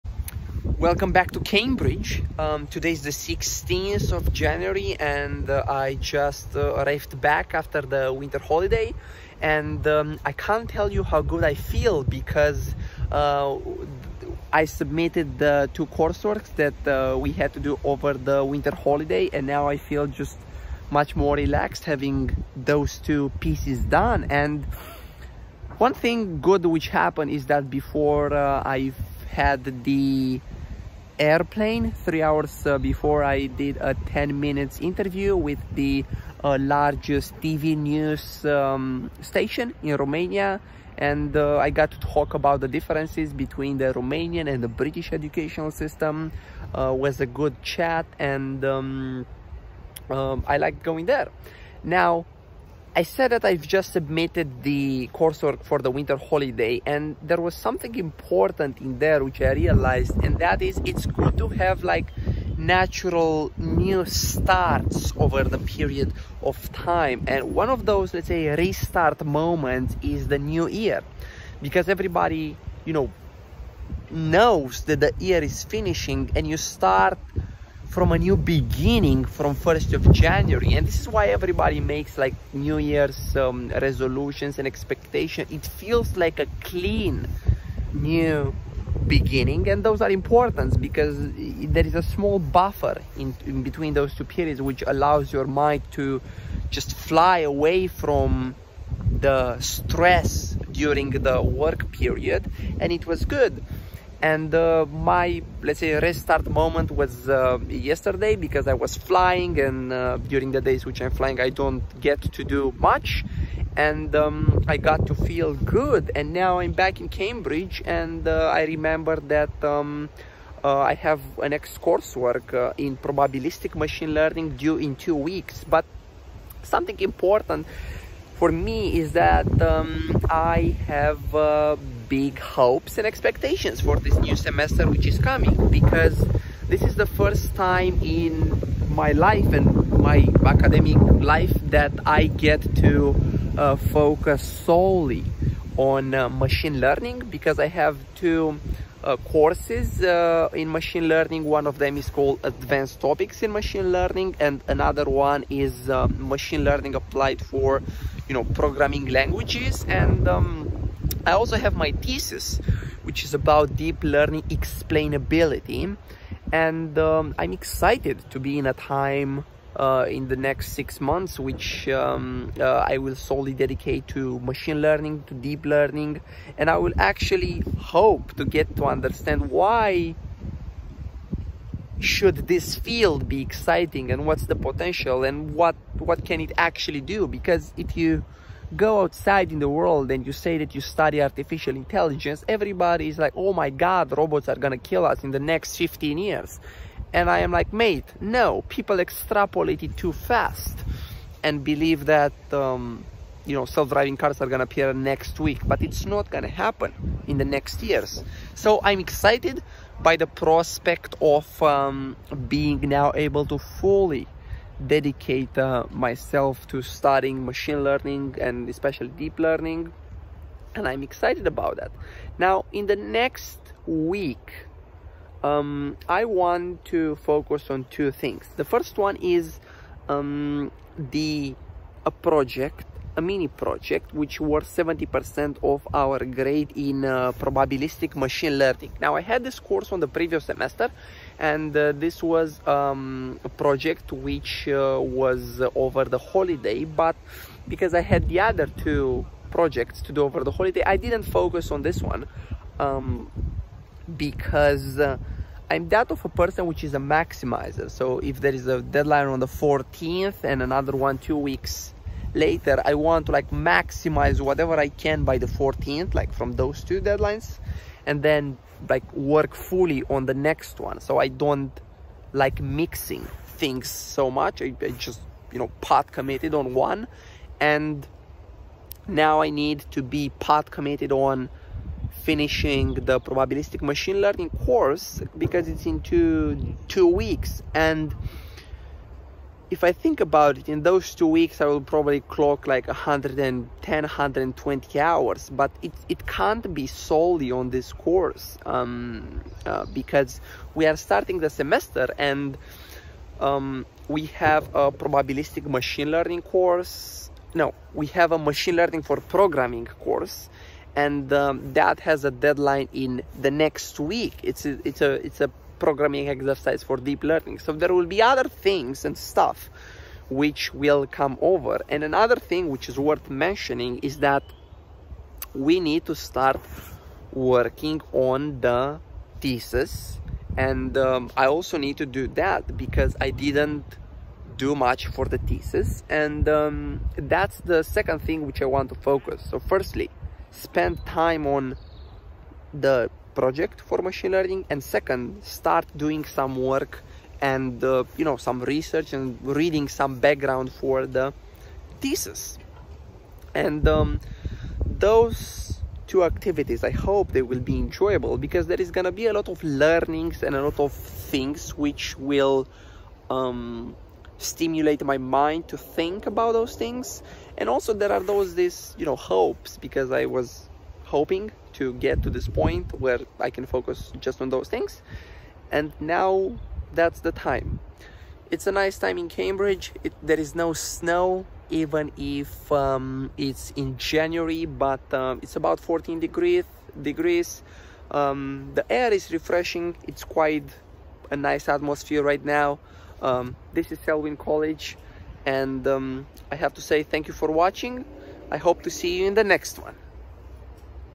Plans for the new semester | Week 14 | MPhil in Advanced Computer Science in Cambridge
I feel released, as I submitted the coursework for the holidays.
Today starts the second semester, and I have high hopes. It's going to be the first time when I can fully dedicate to learning machine learning. Until now, there were always unrelated courses. This semester I take ML courses and the dissertation, and there is no excuse for not making the most out of the coming six months - learning ML.